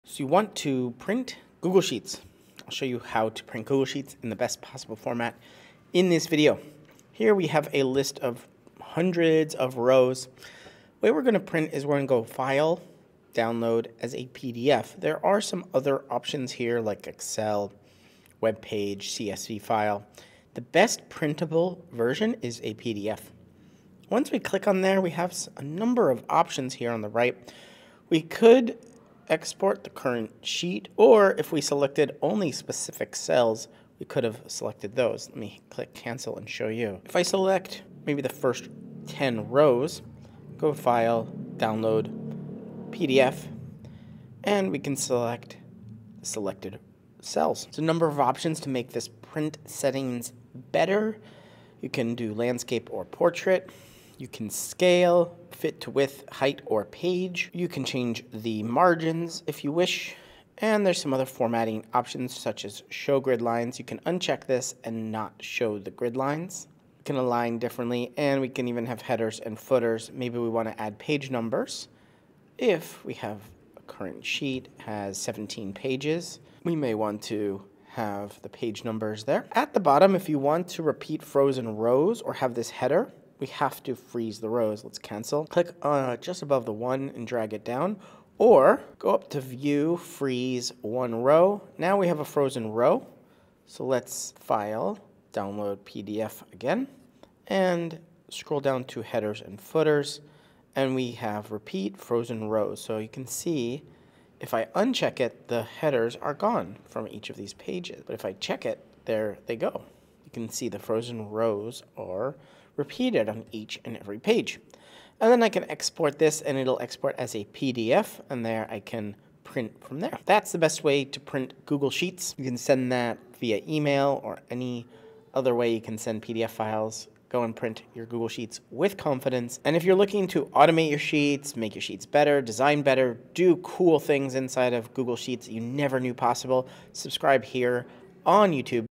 How To Print Google Sheets
How to Print Google Sheets in the Best Format

Learn how to print your Google Sheets in the most optimal format in this step-by-step guide. The tutorial covers downloading your sheet as a PDF, selecting specific cells, adjusting print settings including orientation and margins, and adding headers and footers.

00:00 Introduction to Printing Google Sheets
00:10 Choosing the Best Format for Printing
00:47 Customizing Print Settings
01:10 Adding Headers and Footers
01:25 Freezing Rows for Repeated Headers
01:55 Finalizing and Exporting Your PDF
02:07 More Tips